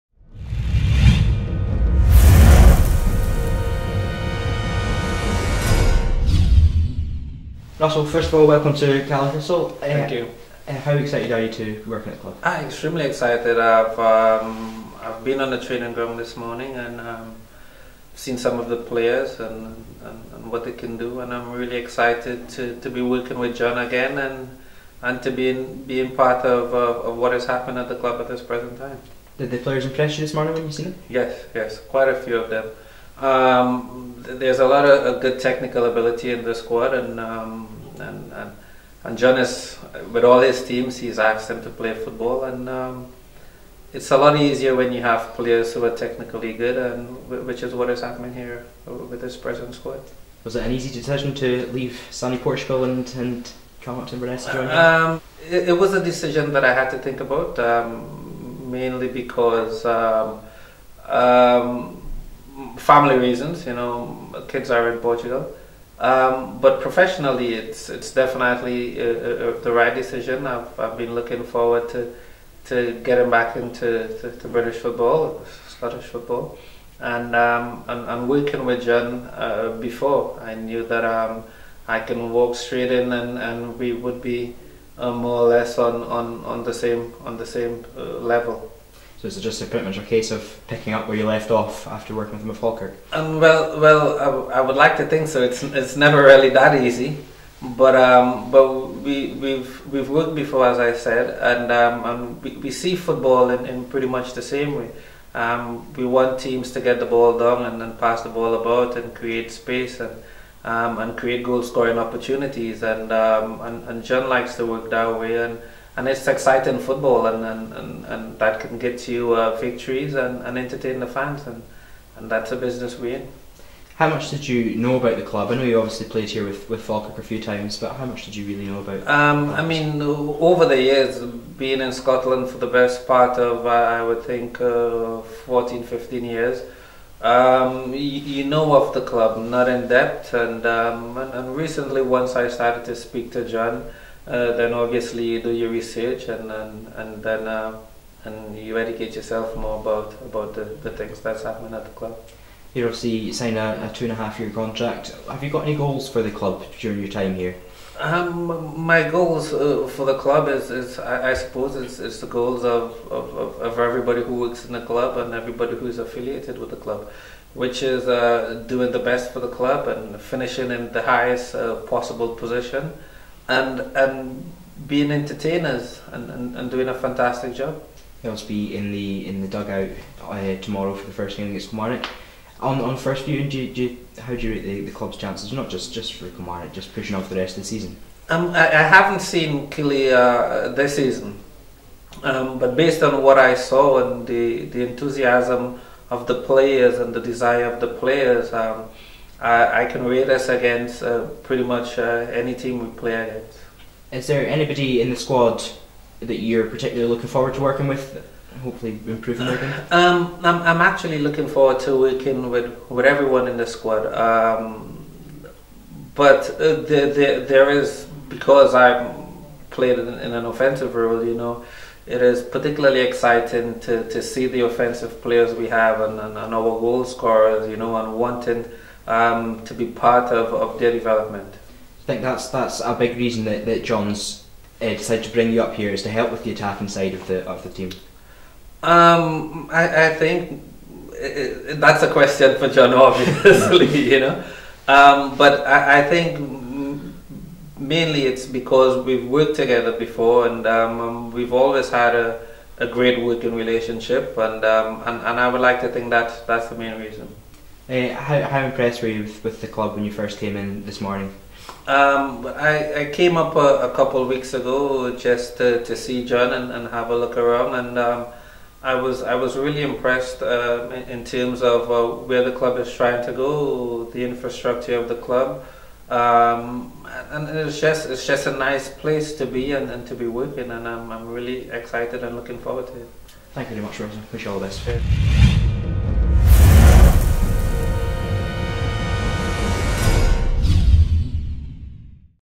Russell Latapy : Exclusive First Interview for Caley Thistle 24/01/14
Russell Latapy has arrived at Inverness Caledonian Thistle.  Grant Pringle caught up with him to chat about his move to Inverness and the first day on the job.